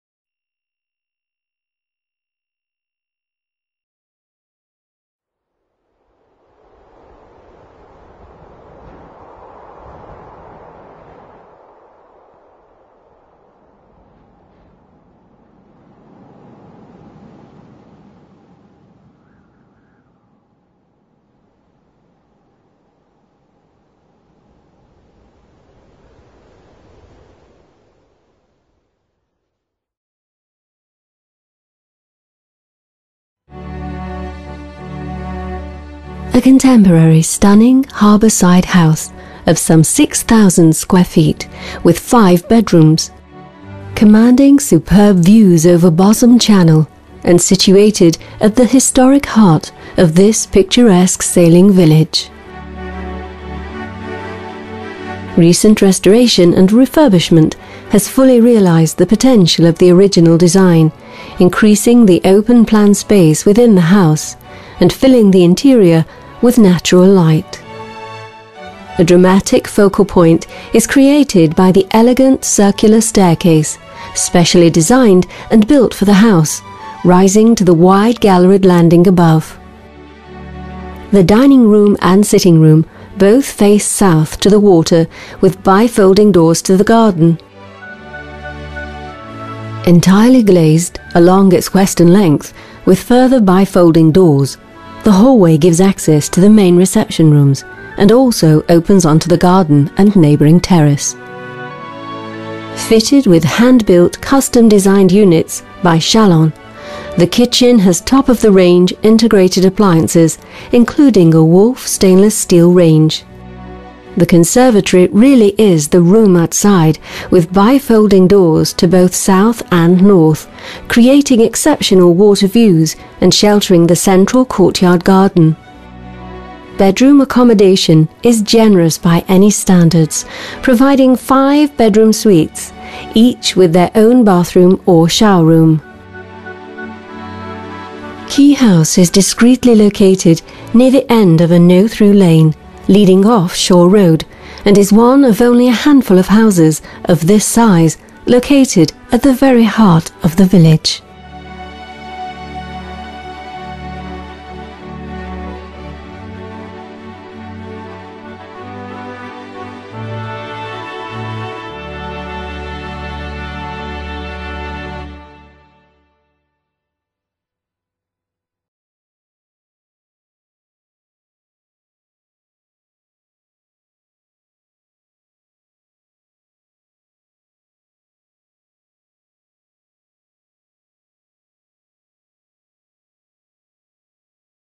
Sailing home on the water, Bosham, for sale.
A stunning contemporary harbourside house of some 6,000 sq.ft. commanding superb views over Bosham Channel and situated at the historic heart of this picturesque sailing village within walking distance of Bosham Sailing Club, High Street and Quay Meadow.